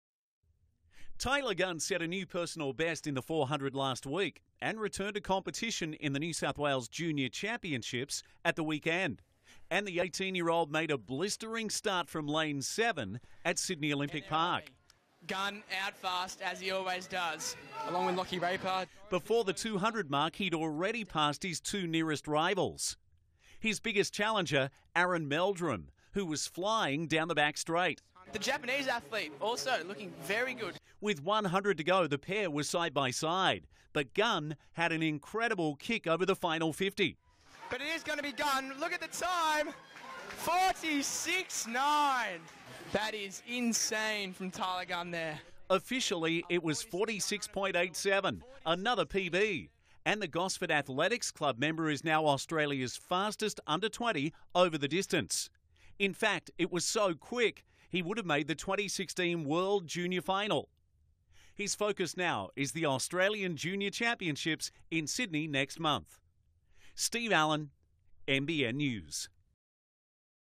Gunn Fires Again to Claim NSW Men's Under 20 400m Title | NBN News
Tyler Gunn claimed the NSW junior championships Under 20 Men's 400m title and set a new Personal Best time of 46.87 seconds. Tyler is currently the fastest Under 20 400m runner in Australia!

Tyler was the 2016/17 Male Athlete of the Year for the Central Coast Academy of Sport.